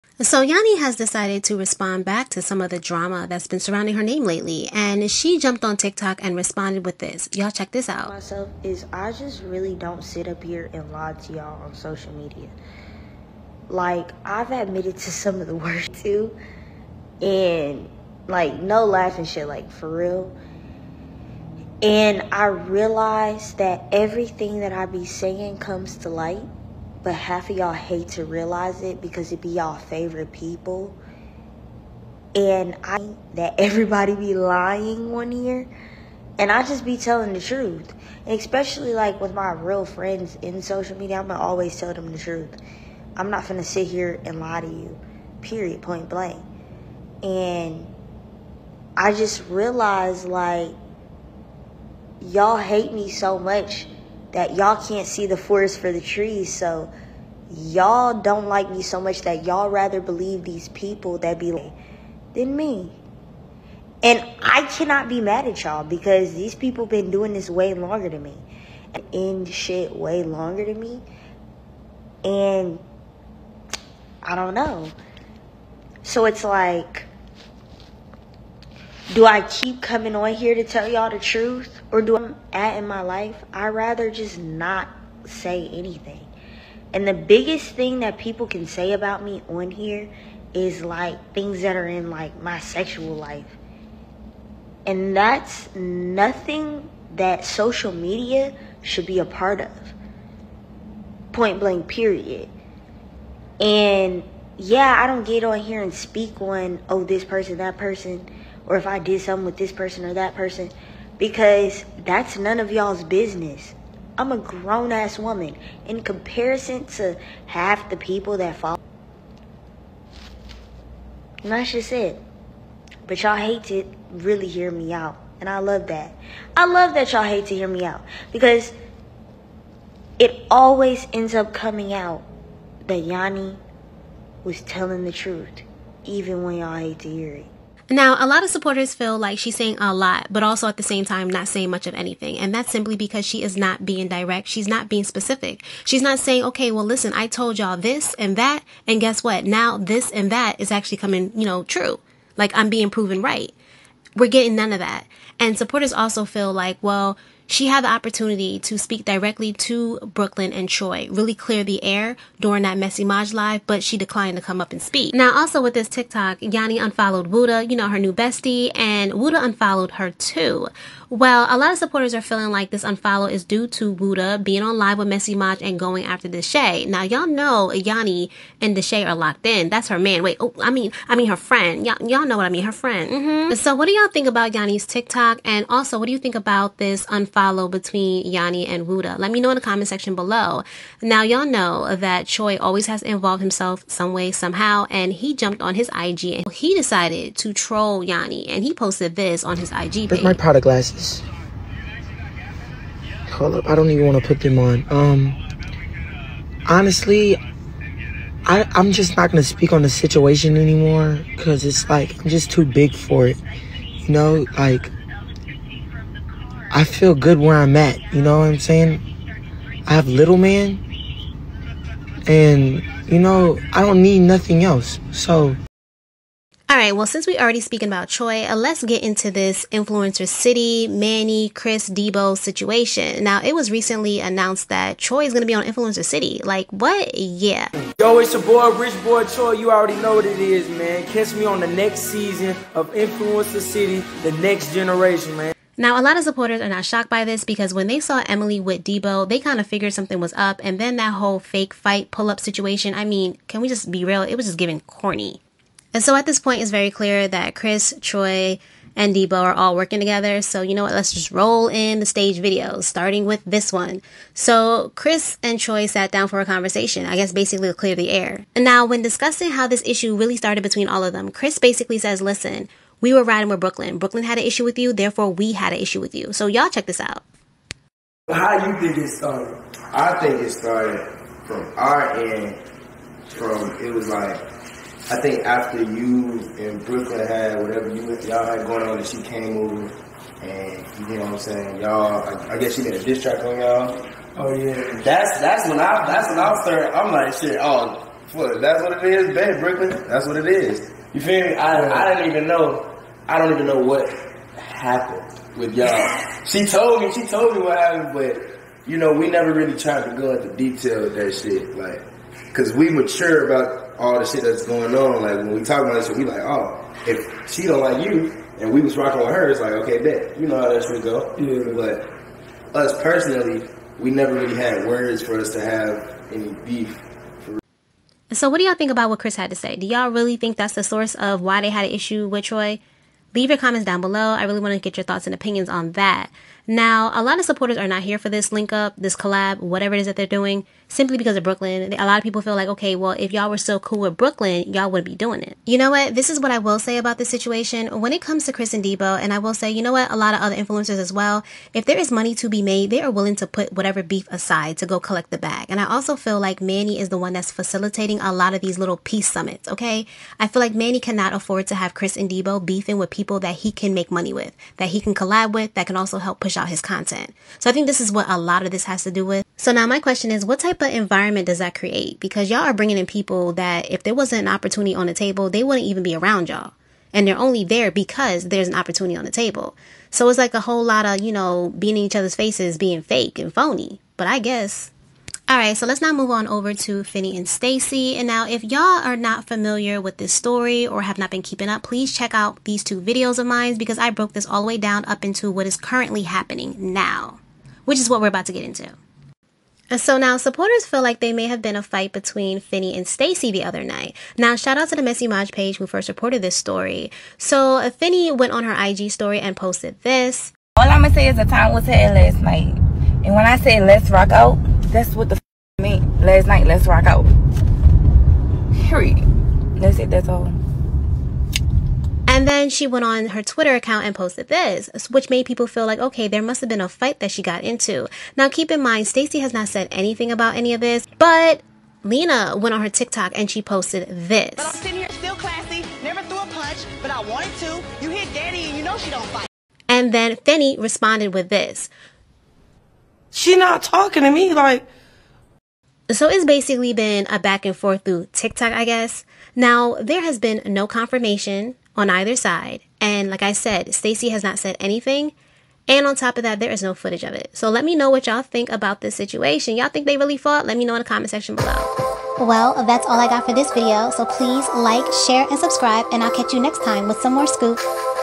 Yanni Responds To The Drama & UNFOLLOWS Wooda! Chris &Troy Squash The Beef!
Yanni Responds To The Drama & Unfollows Wood! Chris & Troy squash the beef!
Yanni responds to all the drama between her and Brooklyn. Chris has a sit down conversation with troy calling a truce, plus more